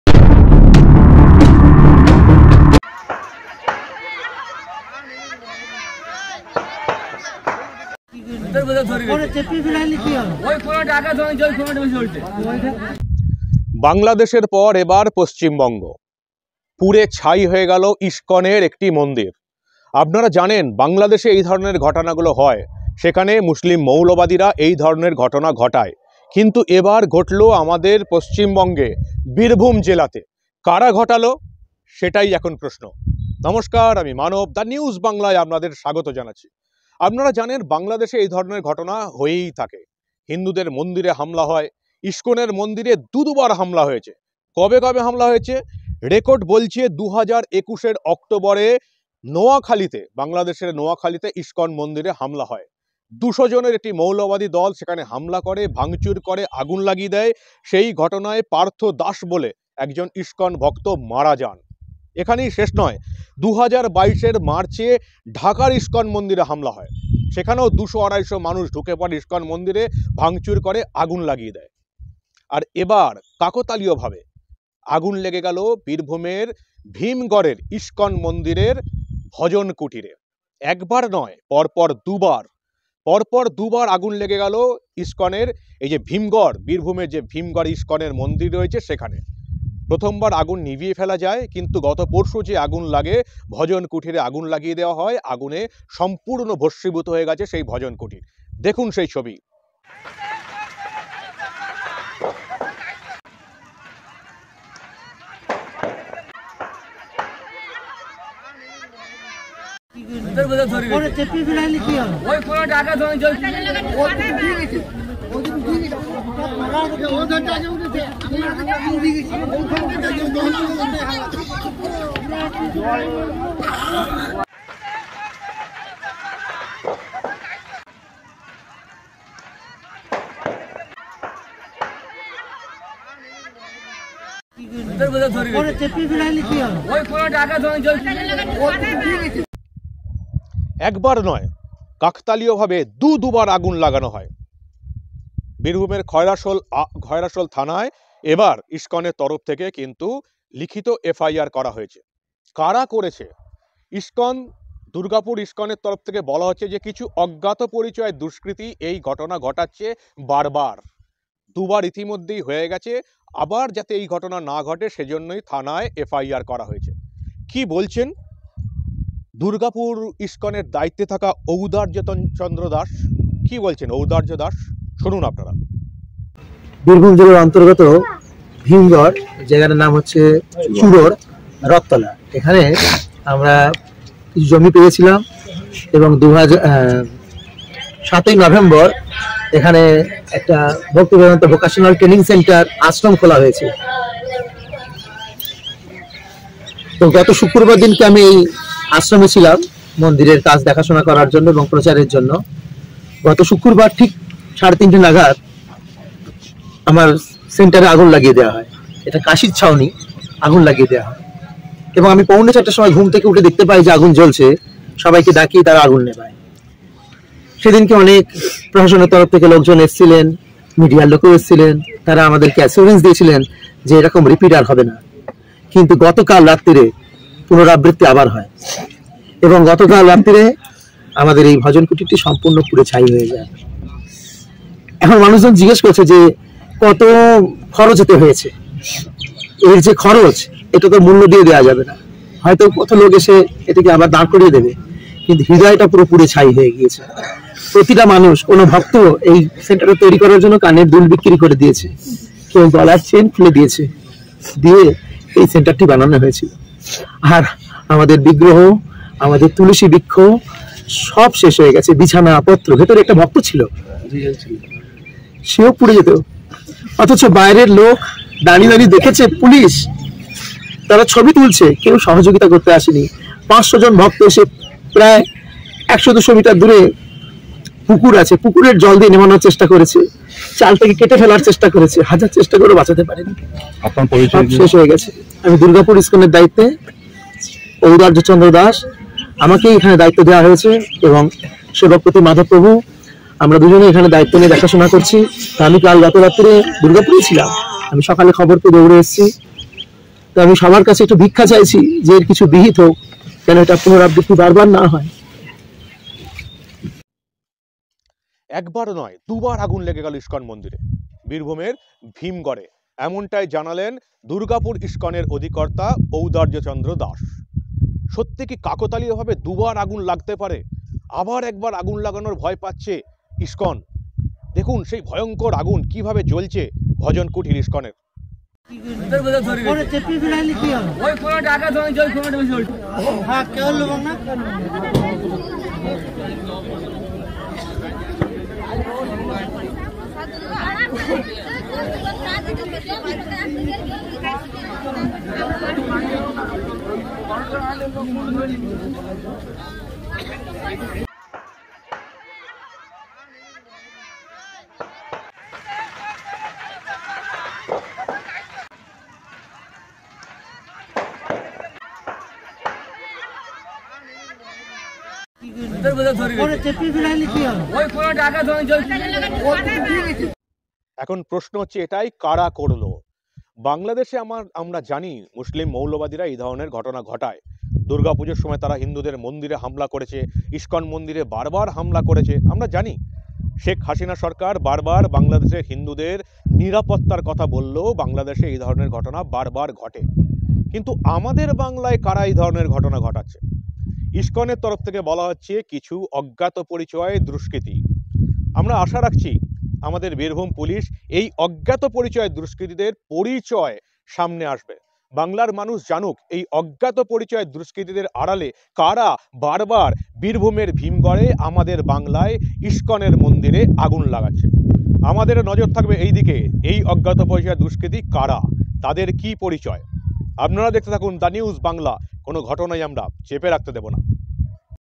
বাংলাদেশের পর পশ্চিমবঙ্গ, ইসকন মন্দিরে আগুন
পুড়ে ছাই বাংলার ইসকন ভজন কুটির @thenewsbangla